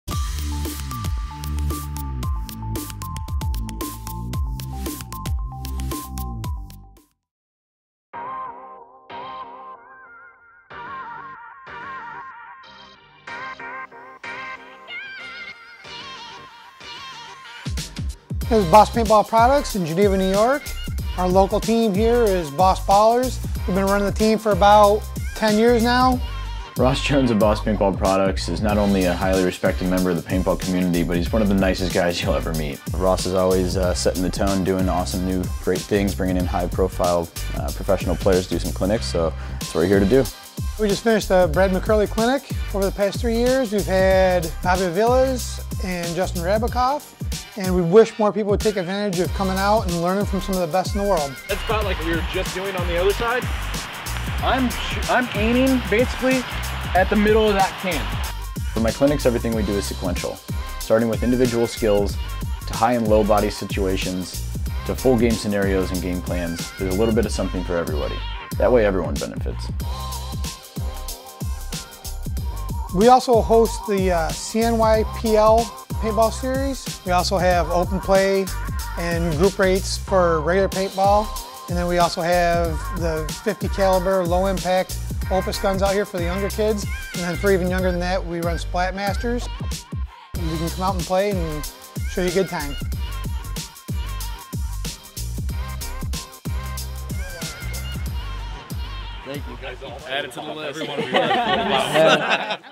Feature Friday - Boss Paintball Products
Ross Jones of Boss Paintball Products has always been a tremendous help as a technician for our staff and sponsored teams - Our feature today is his store and field located in Geneva, New York. BPP annually hosts professional clinics and this year Brad McCurley of ac: Dallas had the chance to improve the skills of their house team, Boss Ballers and other local players. Congrats to the Ballers for winning their event in the NEPL event the day after the clinic! // bosspaintballproducts.com